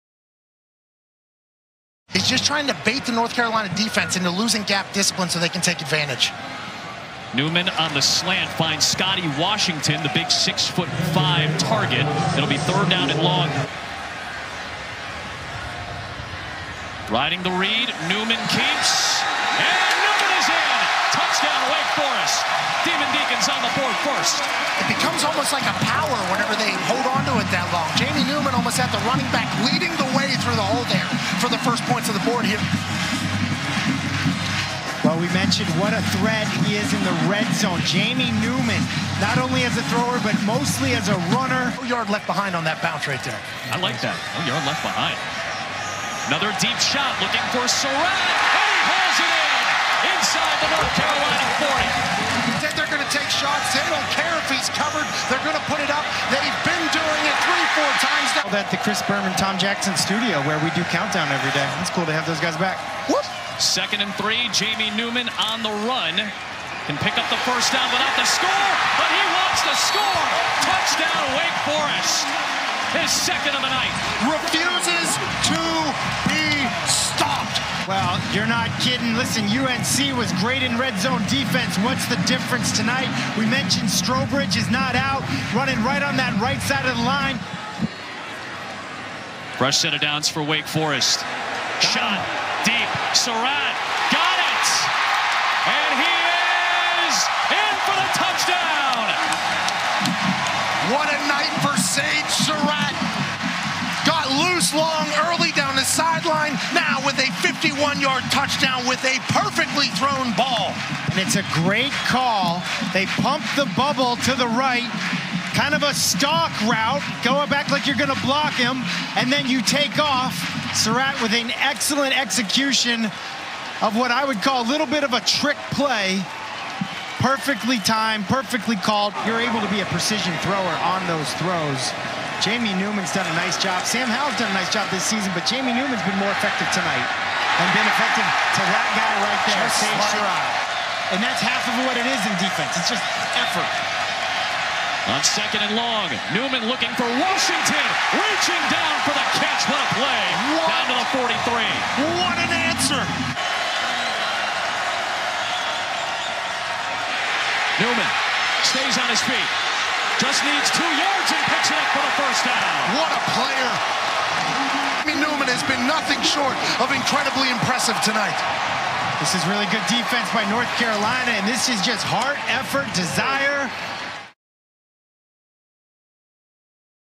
Another Big Night For Wake Forest's Jamie Newman Against UNC
Wake Forest quarterback Jamie Newman has been one of the big stories of the ACC so far this season. He had another big game against North Carolina going 14-26 for 214 yards through the air, running for 78 yards, and scoring a total of three touchdowns in the game.